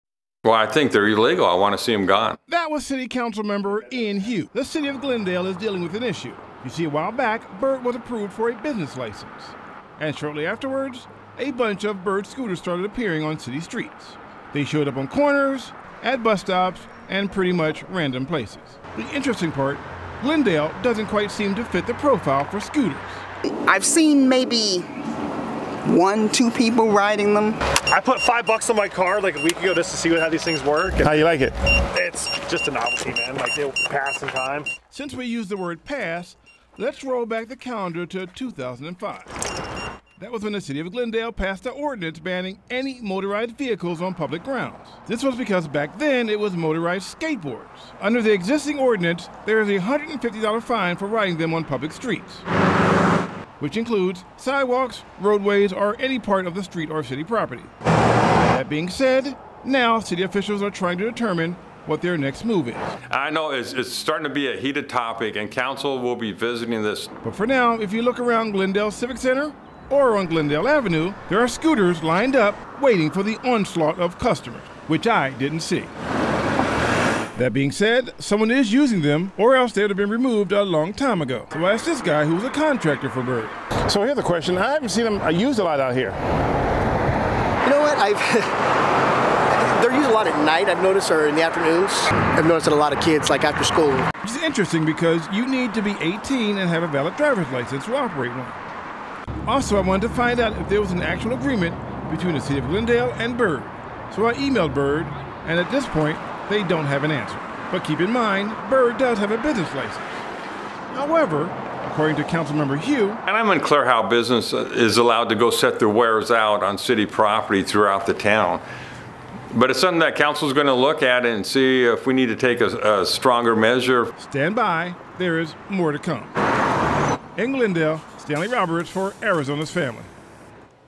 Glendale councilman upset over electric scooters
A Glendale councilman thinks electric scooters might be illegal.